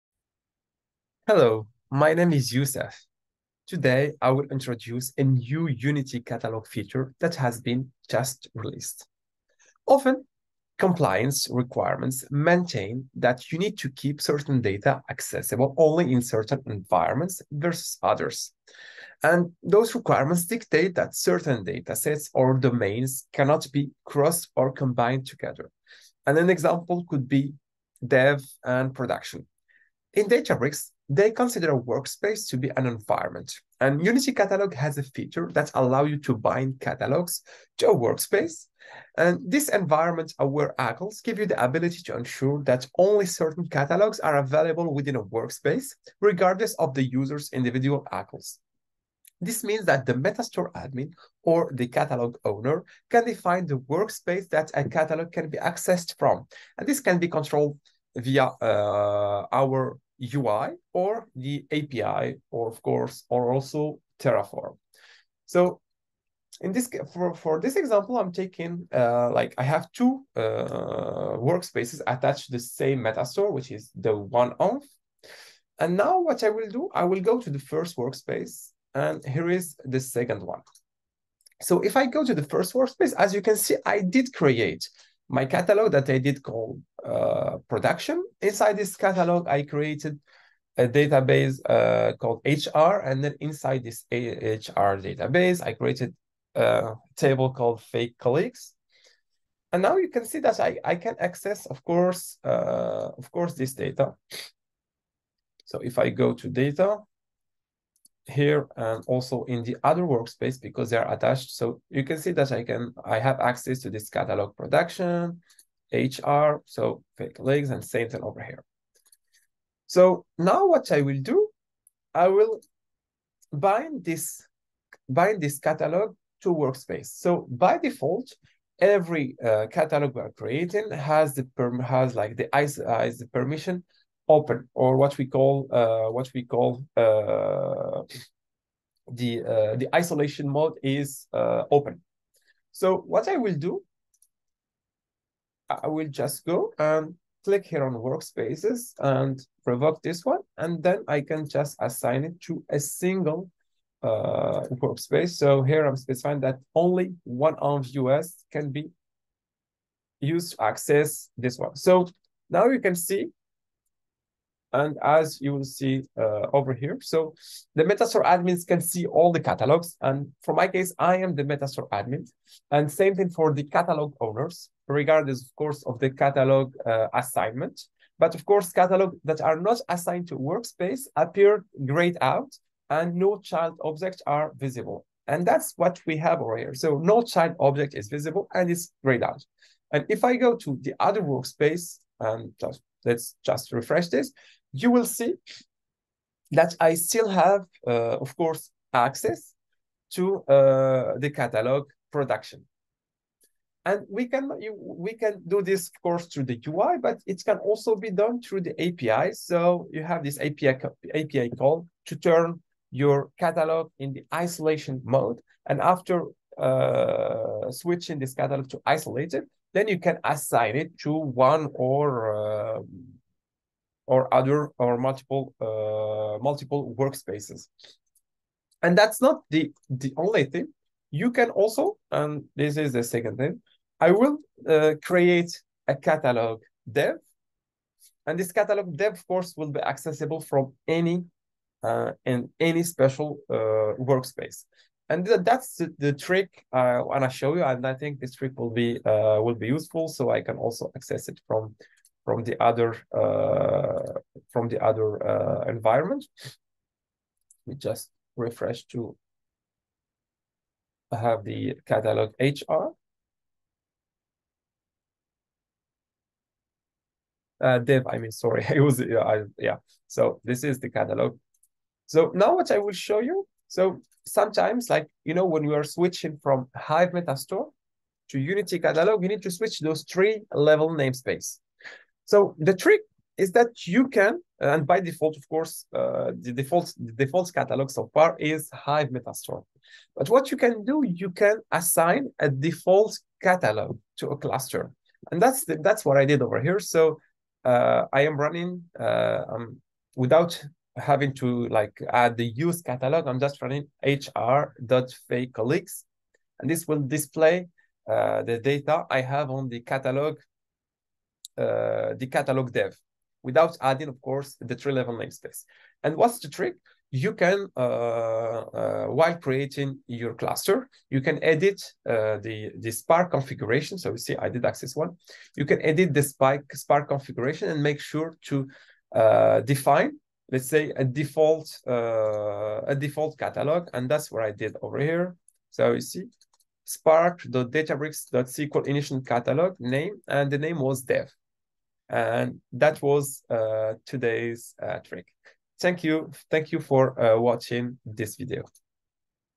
Getting started with Catalog binding in Unity Catalog
Chapters
• 0:00 - Introduction to Unity Catalog Binding for Data Isolation and Compliance
• 1:30 - How Catalog Binding Works (Workspaces as Environments, Control via UI/API/Terraform)
• 3:00 - Demo: Binding a Catalog to a Specific Workspace
• 4:30 - Trick: Setting a Default Catalog for a Cluster